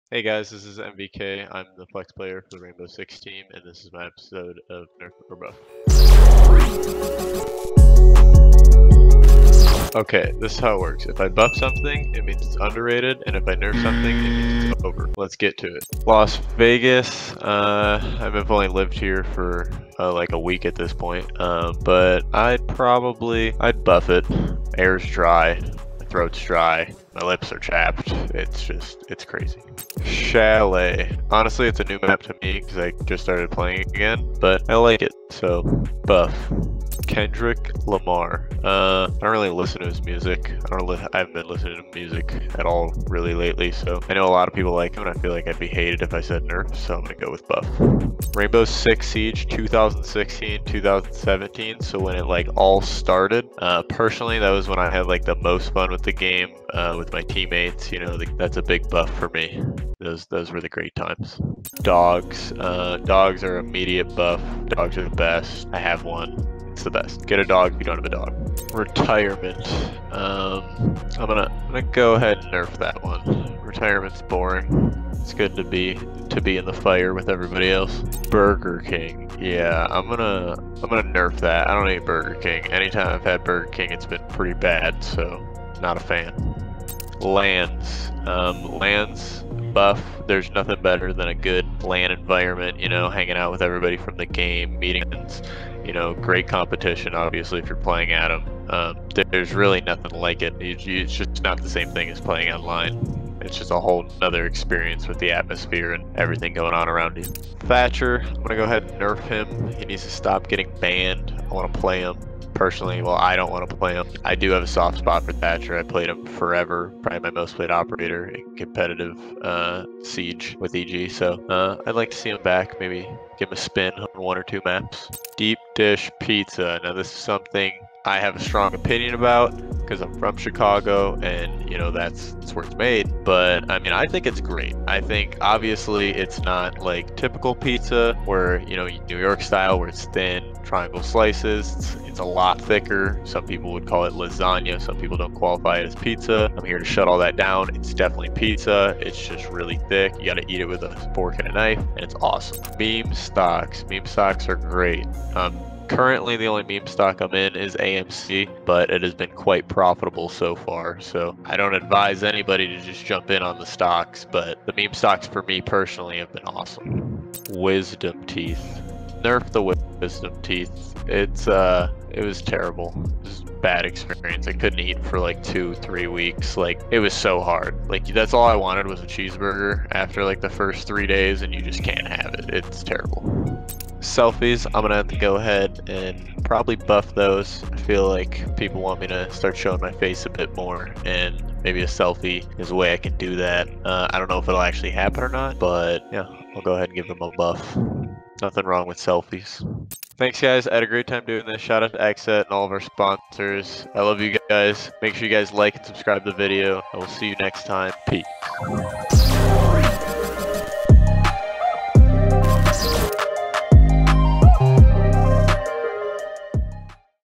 Meme stocks are underrated? 🤔 (NerfOrBuff) ft. XSET nvK
LANs 👍
Retirement 👎
Deep Dish Pizza 😳

Get more of nvK's hot takes this week on NerfOrBuff! 👀